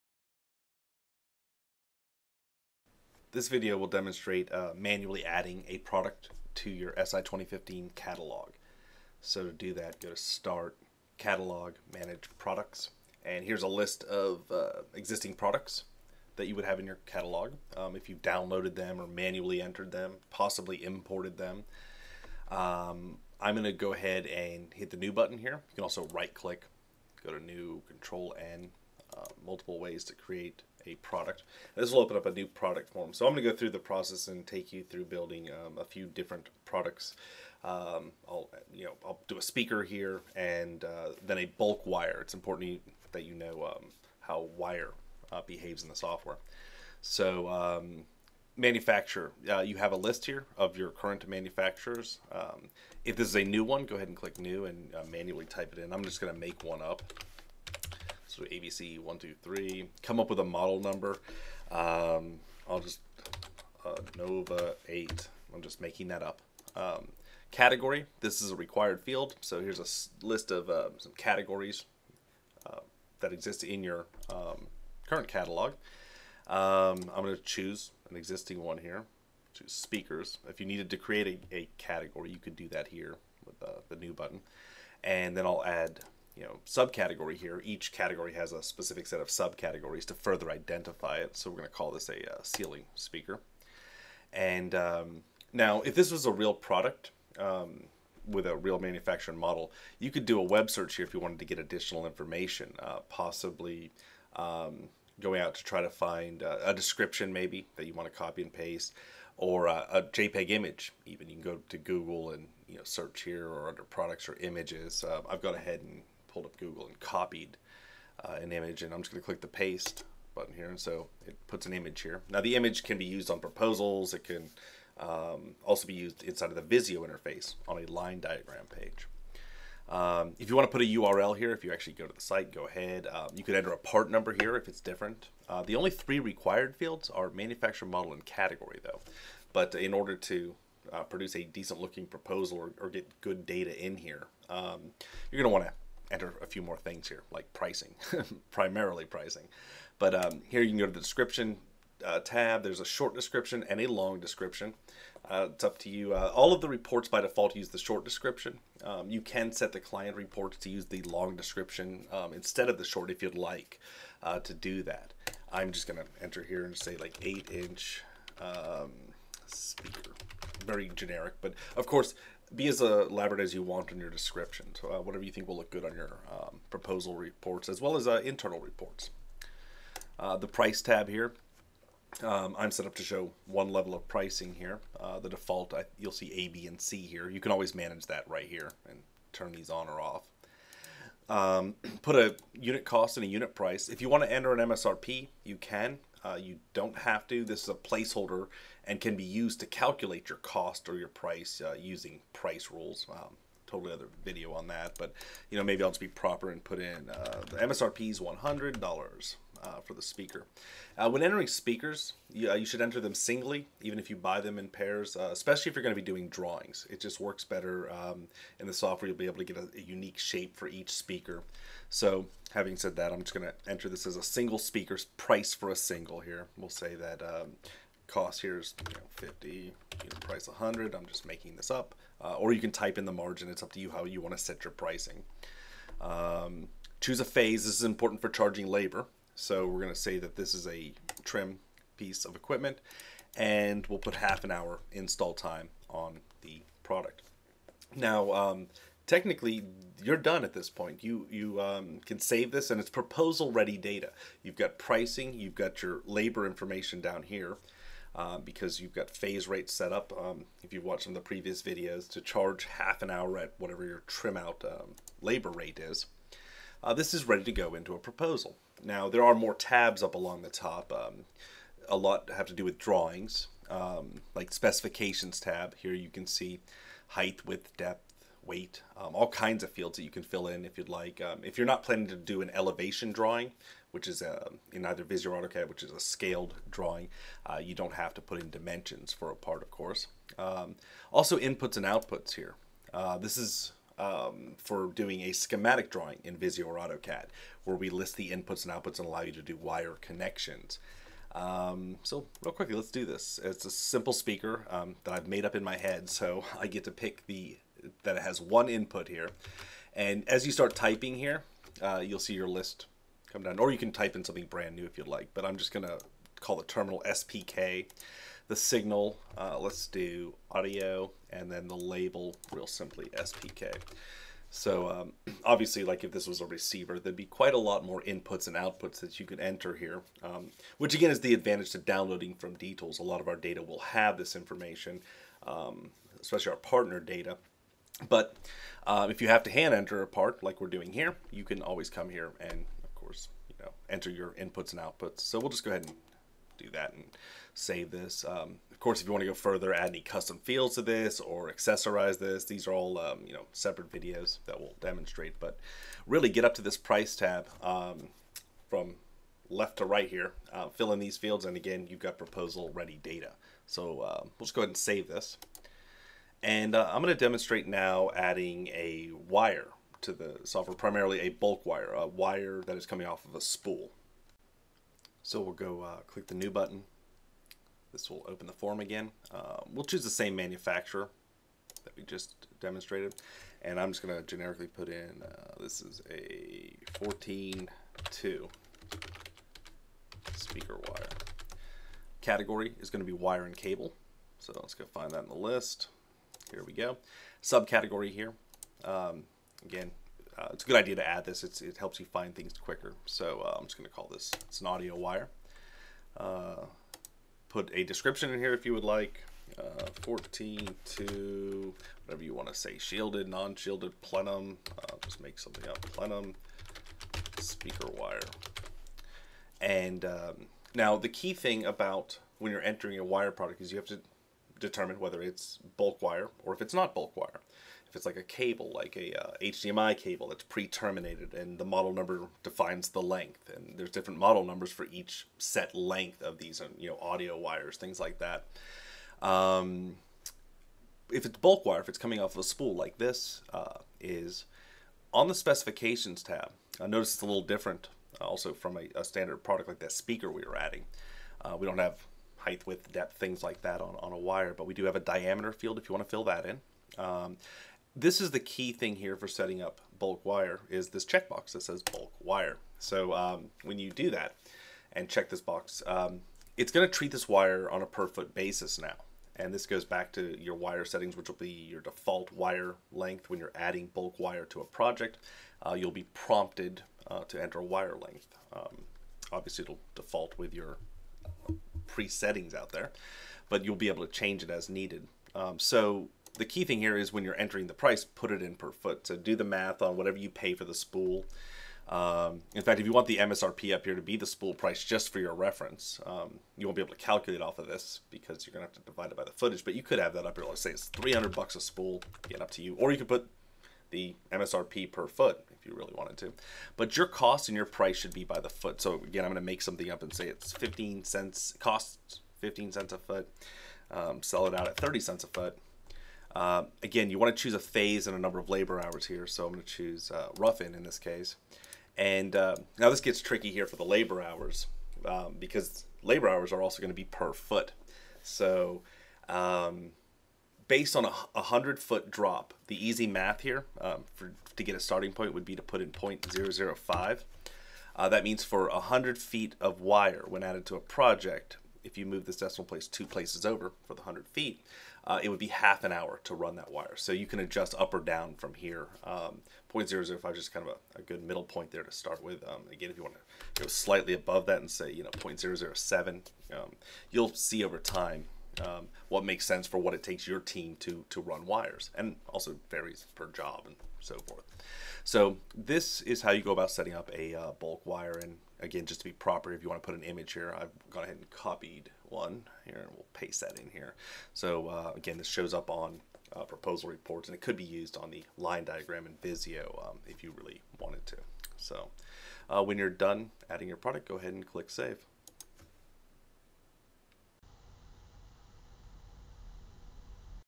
Adding Products - New Button
This video demonstrates manually adding a Product to your Catalog in D-Tools SI 2015.